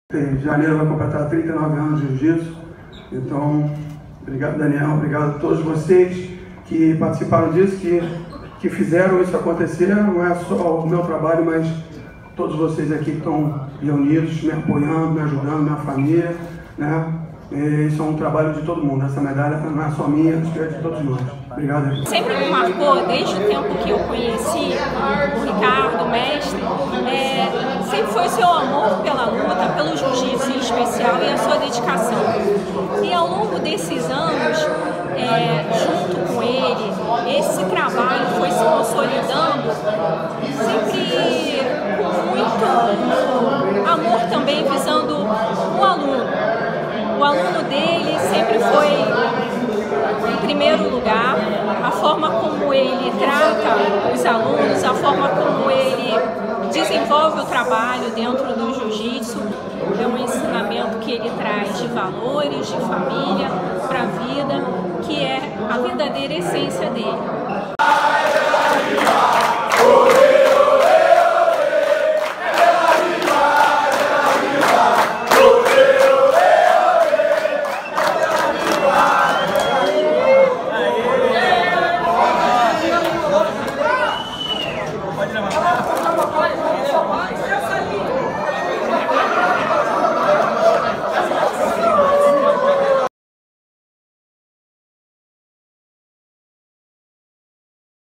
Ricardo de la Riva - Delariva Jiu-jtsu - Destinos do Rio
A história bem sucedida de Ricardo de la Riva no Jiu Jitsu na coluna de Leonardo Saboia na Destinos do Rio